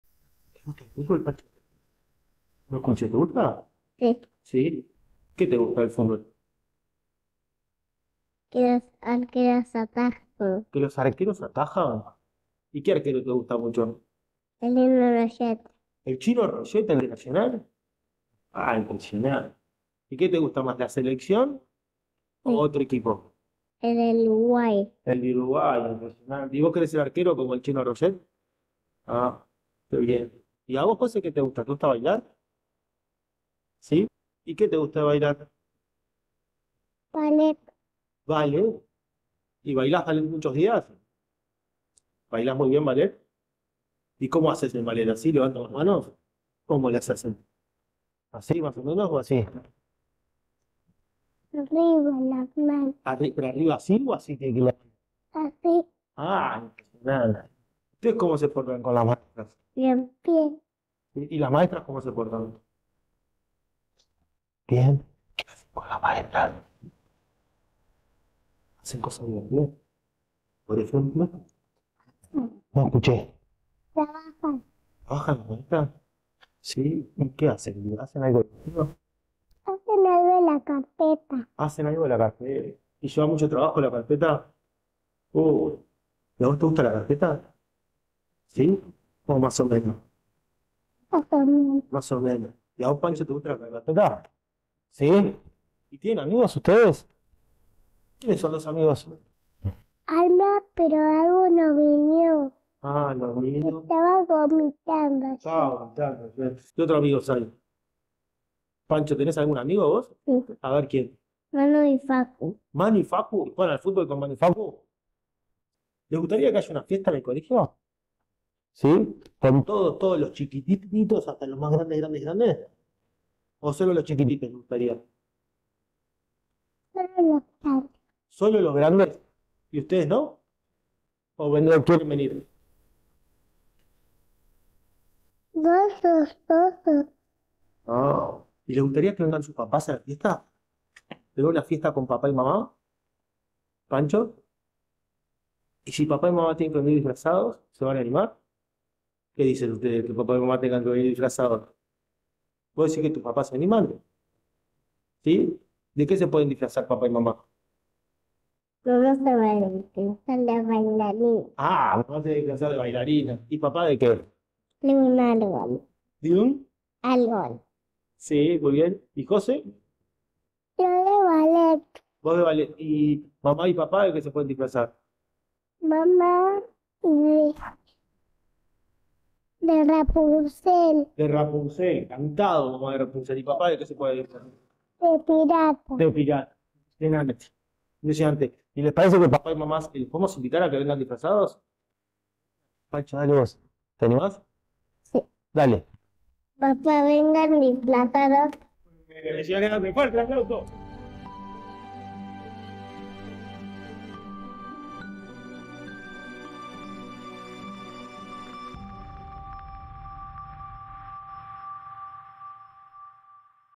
Entrevista a Jose y Panchito (¡Se viene la Safada!)
En el mes de la SAFADA compartimos una interesante charla con Jose y Panchito, de Educación Inicial.